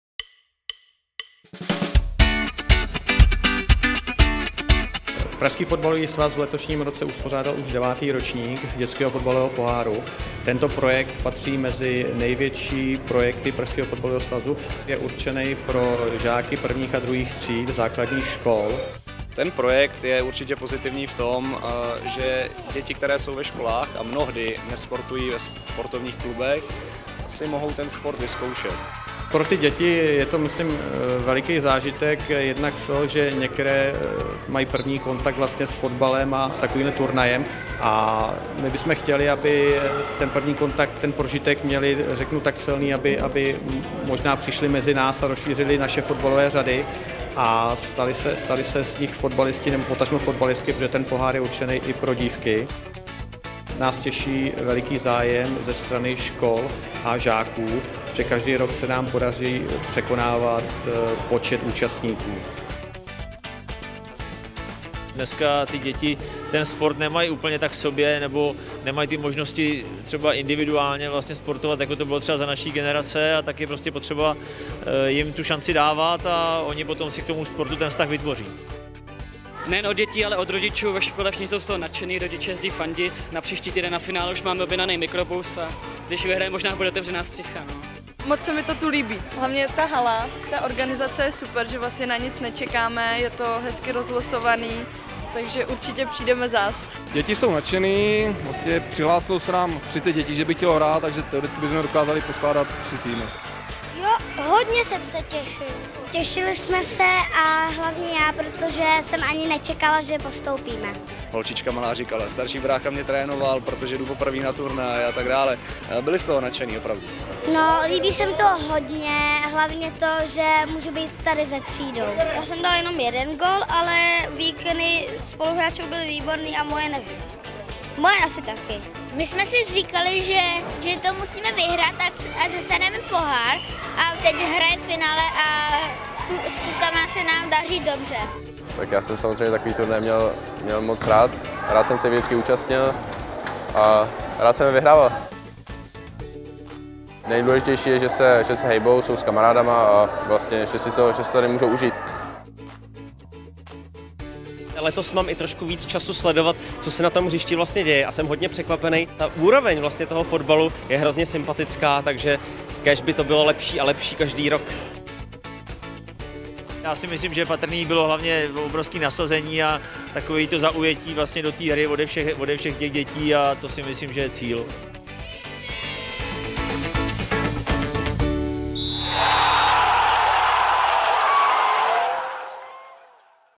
Dětský fotbalový pohár 2019
Rekordní počet týmů, rekordní počet turnajů - takový byl Dětský fotbalový pohár 2019. 🏆 Podívejte se, jak DFP vypadal pohledem kamer a co na něj říkají Václav Drchal a Luboš Kozel. 🎦